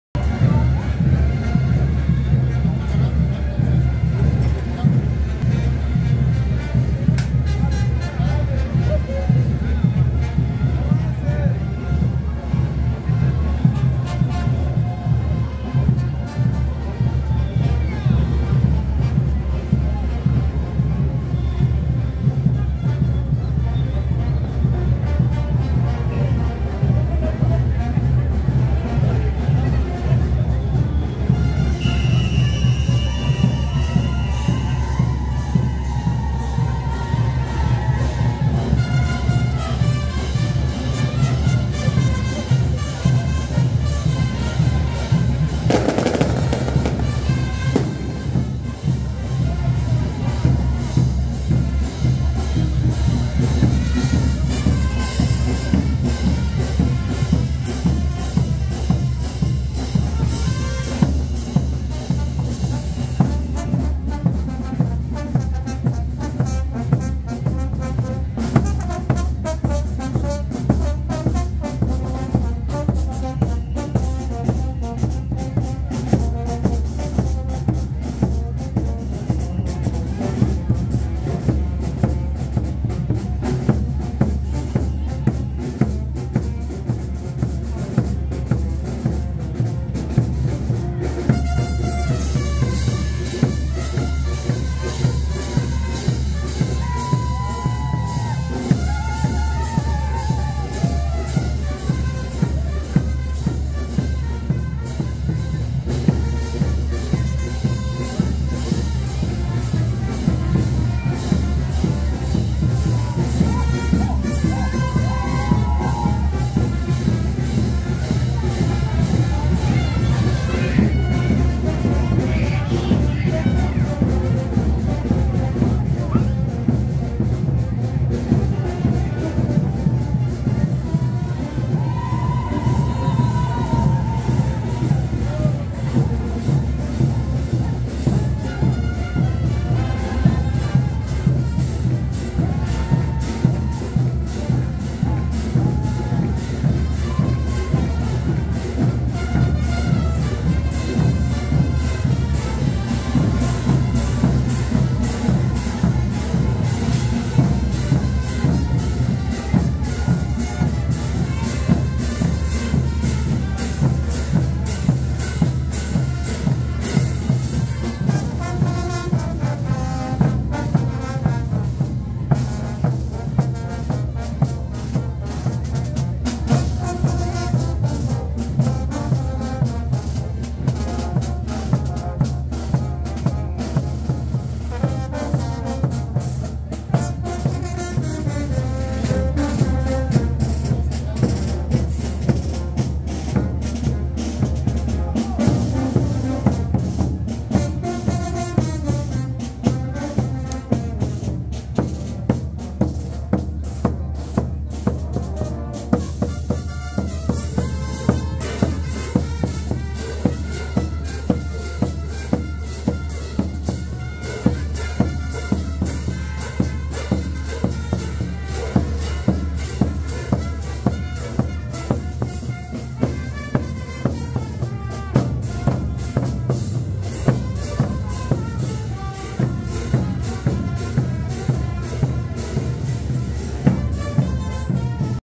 Gran Poder 2022 La Promesa: Wacas Tocoris Aymaras de Bolivia
El Paso de los Wacas Tocoris Aymaras de Bolivia en La Promesa del Gran Poder 2022, Parte 2